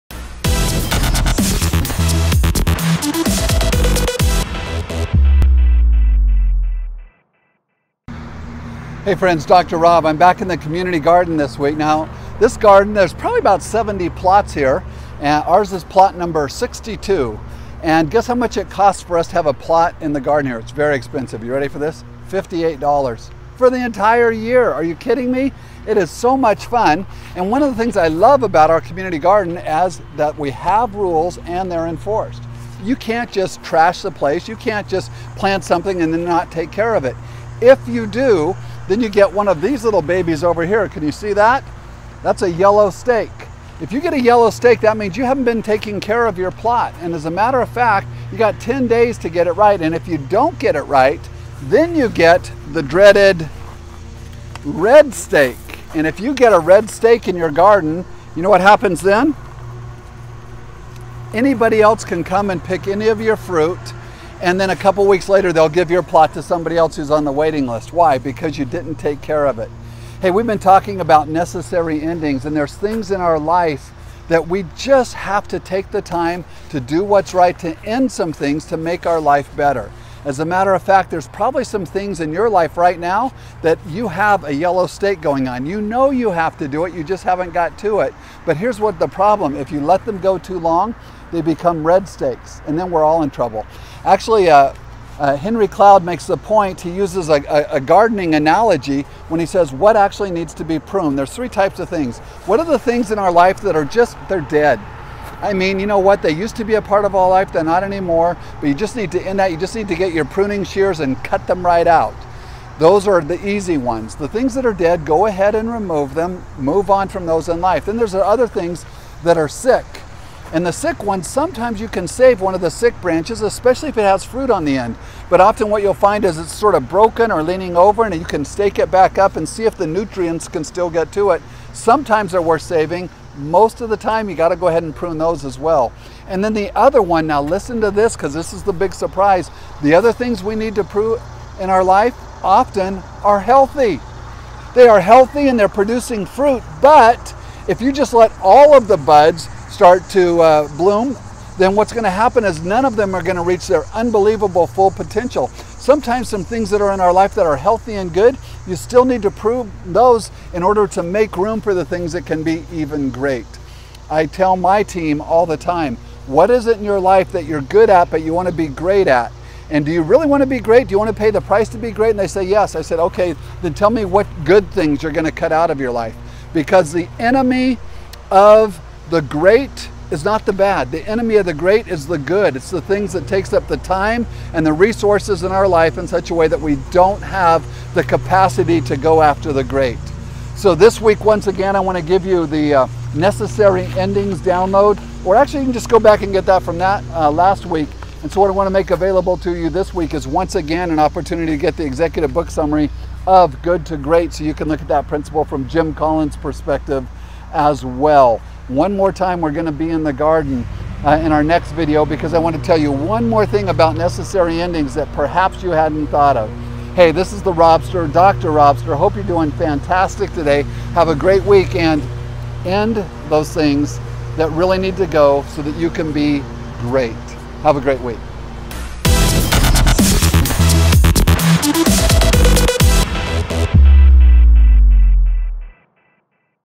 Go From Good To Great By Removing These 3 Things - LeaderTribe.com
The first two make sense, but the 3rd one is the game changer

Last week, taking research from Henry Cloud's book Necessary Endings we talked about preparing the soil of your life. We're upping the ante this week with some very practical research telling you exacting what you need to prune in order to go from Good to Great.

"Wait, Robster. Did you say, Good to Great? Like the title of the book?"

Because the 3rd (and most painful) area that needs to be pruned, has everything to do with moving from good to great in your life.

The first area of pruning is cutting the "dead branches" out of our lives.  These are the items that are taking up space (physical space or mental or emotional energy) that we need to sever once and for all. Here's a great hint at how to get rid of things that need to go: Stop asking, "What should I get rid of?" That question isn't always clear and we hold on to too many things. Instead ask, "What is it absolutely necessary that I keep?" And then get rid of everything else. It's amazing the freedom that awaits you if you apply that principle.

Second, we must prune the sick branches that are not going to get well. If things are getting better, you might want to give them a little more time.Make the adult decision to get rid of half-baked ideas and the half completed projects that weigh you down.

Finally, and this is the shocker, you must get rid of the healthy branches that are not the right ones. Jim Collins notes that so few people live GREAT lives because it is fairly easy to live a good life. They don't want to get rid of anything good, so they'll never hav the capacity to go for the great.

You have a chance to live GREAT this week. Go for it. Watch the video to see how.